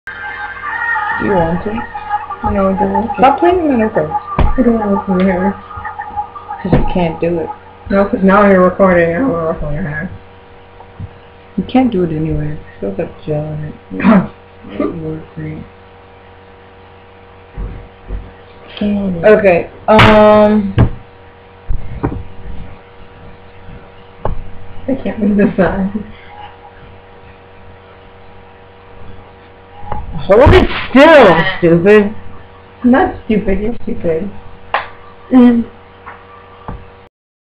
The camera kept recording after I had ended the video
my sis and i were messing around and the camera was still on.